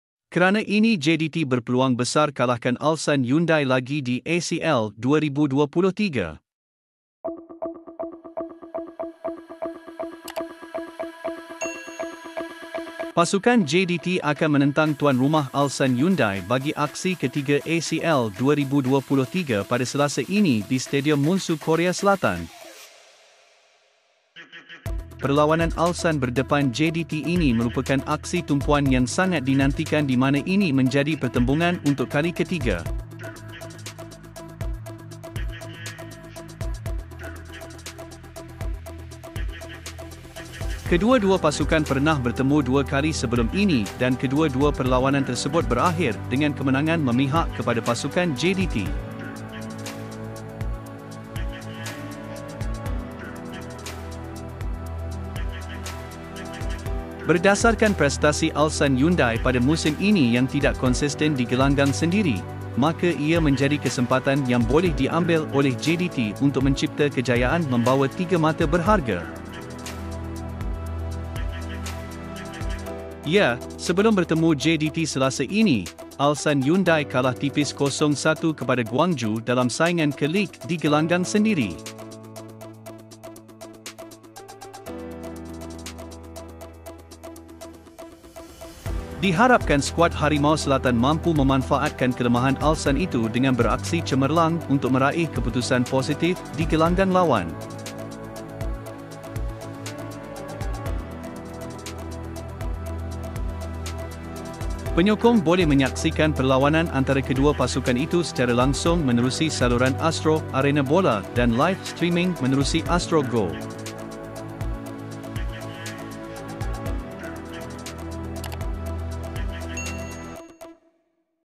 Kerana INI! JDT Berpeluang BESAR Kalahkan Ulsan Hyundai di ACL 2023
ACL 2023 JDT Menentang Ulsan Hyundai! Peluang Johor Darul Ta'zim untuk MENANG! Pasukan JDT akan menentang tuan rumah Ulsan Hyundai bagi aksi ketiga AFC Champions League 2023 pada Selasa ini di Stadium Munsu Korea Selatan.